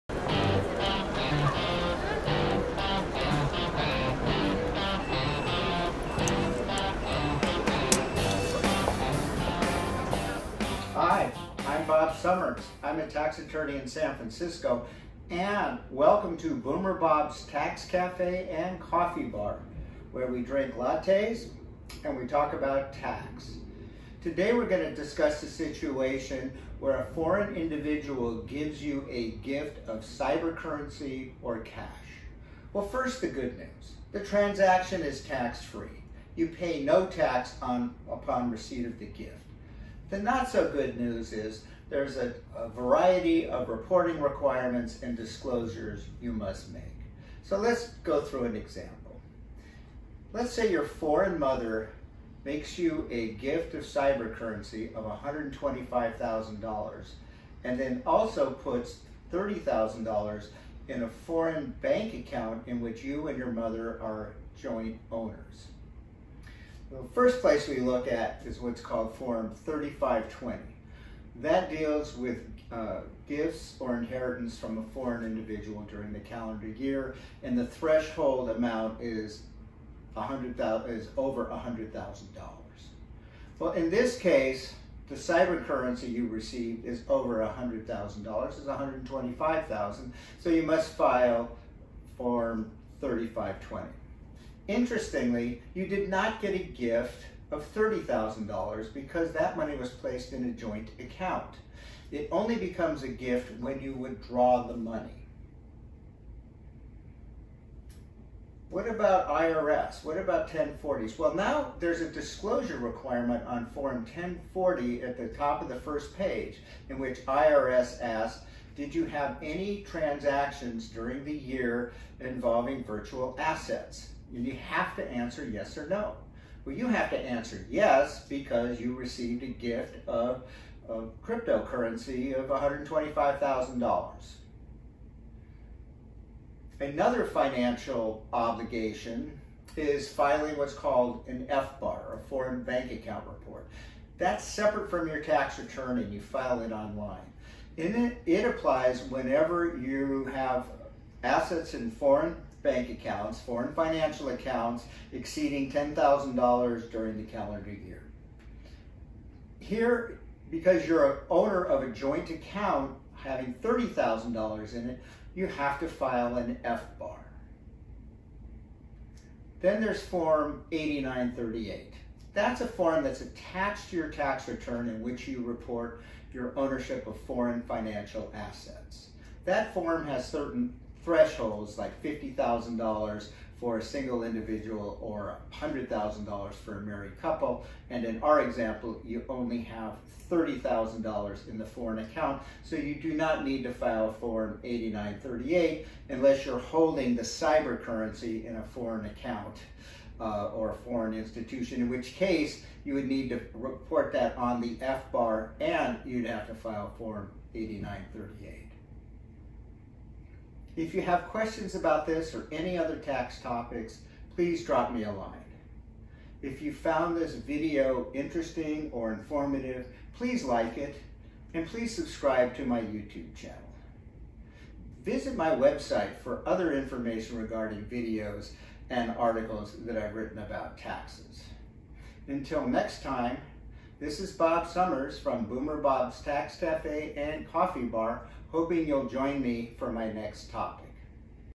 Form 3520: Foreign Gift of Cryptocurrency or Cash
Say you receive a gift or inheritance from a foreign individual of cryptocurrency, or other assets, including cash: First, the good news – the gift is not taxable to either the foreign donor (giver) or the done (recipient) – you receive the gift tax free. The bad news, check out these potential filing and disclosure requirements (including Form 3520)!